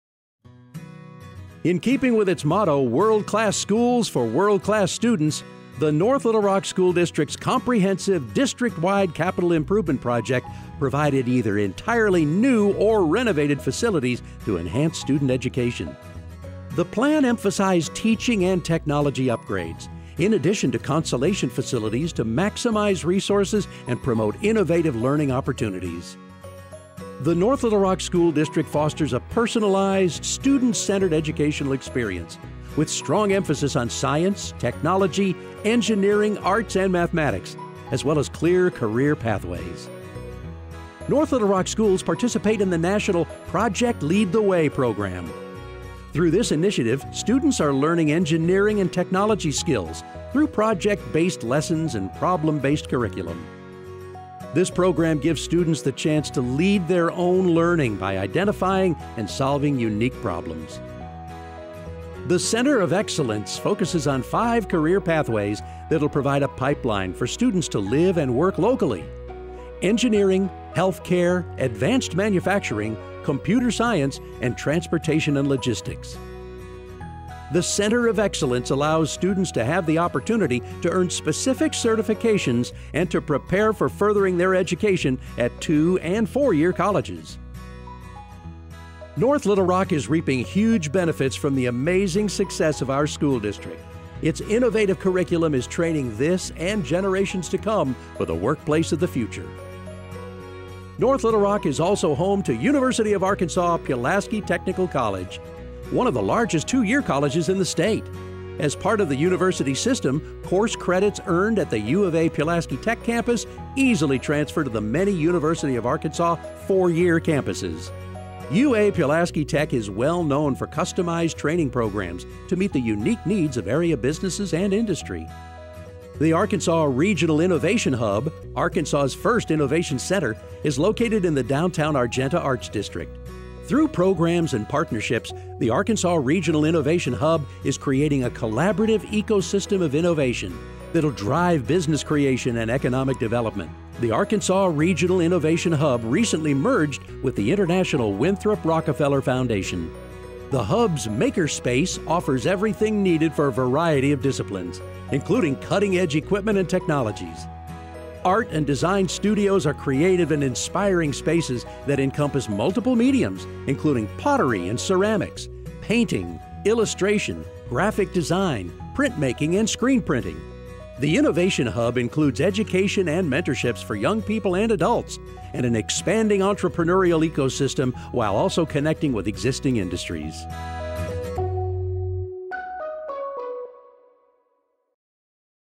NLR Education
This is a promo video I did for the City of North Little Rock, AR